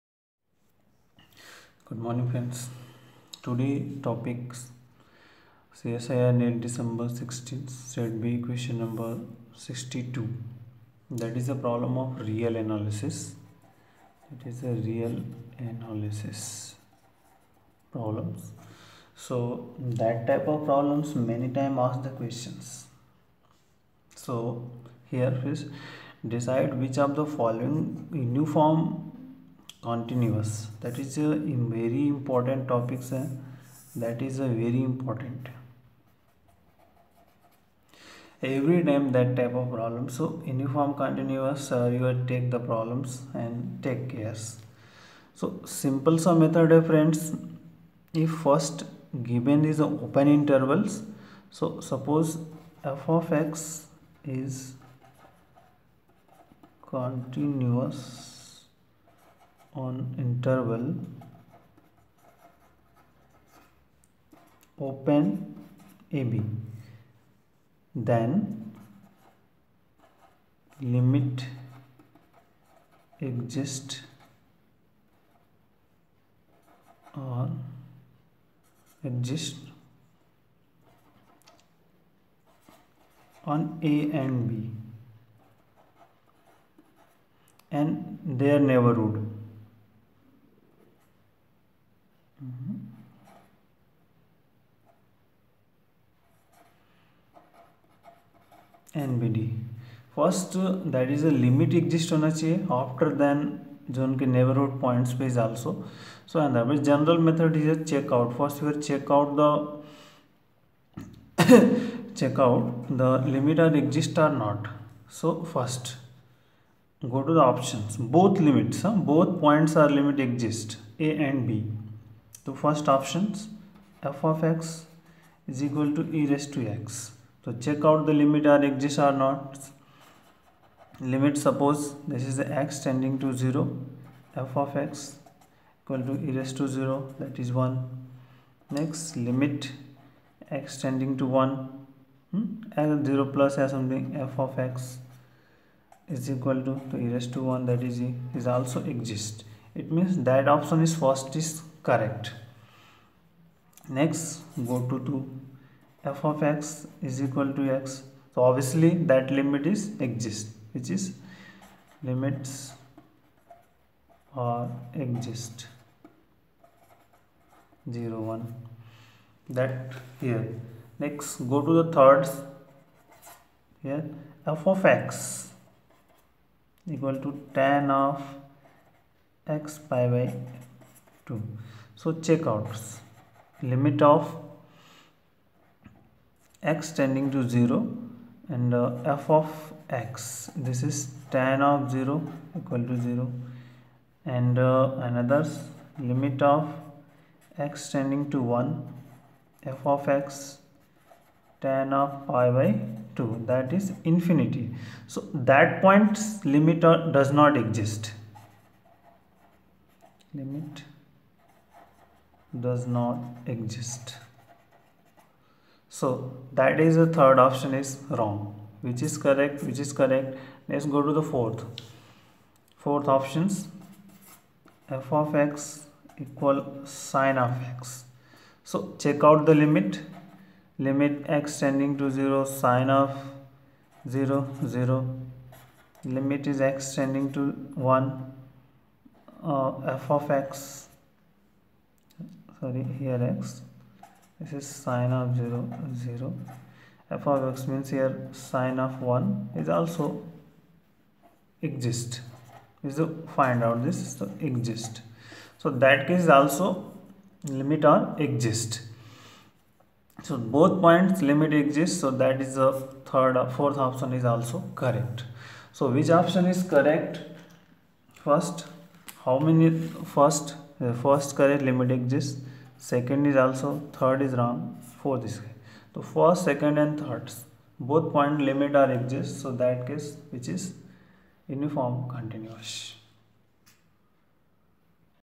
CSIR NET DEC-2016 Q.62 MATHS SOLUTION | Real Analysis -Uniform Continuous Function
Csir net dec 2016 q.no.62-real analysis |uniform continuous,